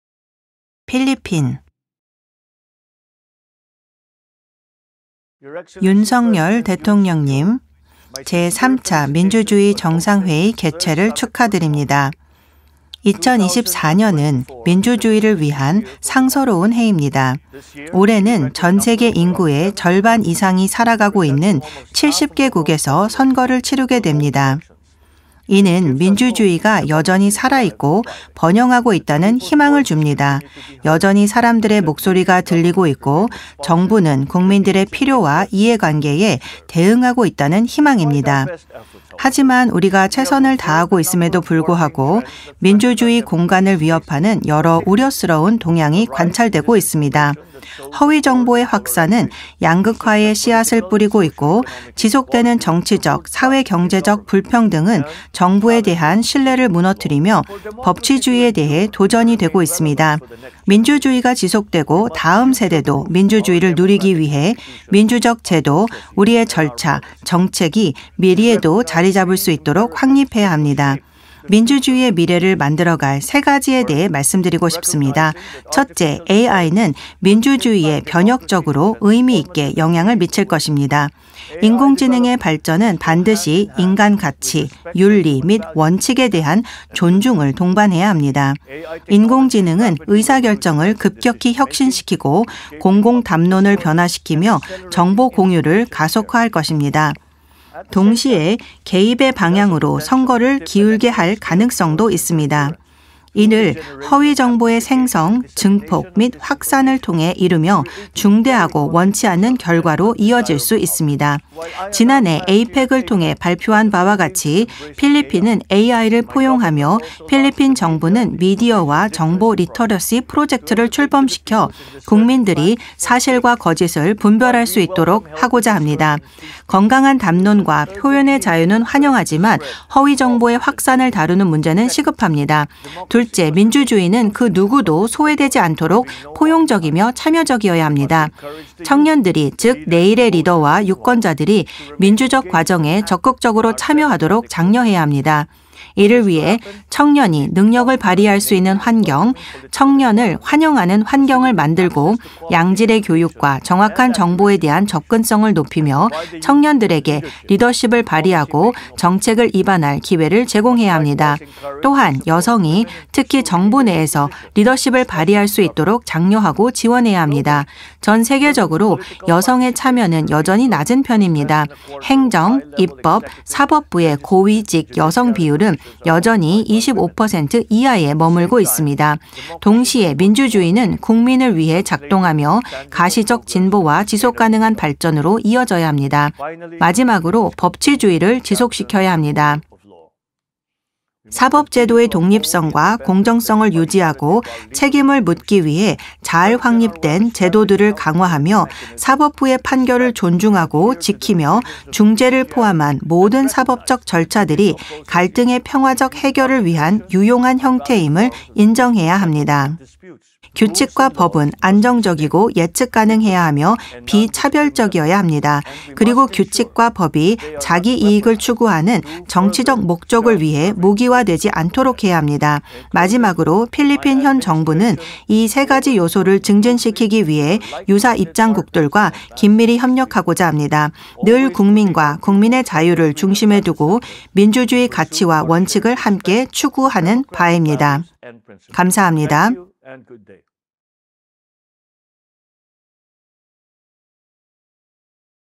제3차 민주주의 정상회의 국별공약영상 필리핀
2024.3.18.~20.간 ‘미래세대를 위한 민주주의’를 주제로 제3차 민주주의 정상회의가 개최되었습니다. 동 정상회의의 일환으로 참석국들은 지난 제1·2차 민주주의 정상회의 이후 공약 이행 진전 상황, 코호트 활동, 그리고 신규 공약 등을 담아 국별공약영상을 제출하여 민주주의 증진을 위한 그간의 노력과 앞으로의 의지를 보여주었습니다.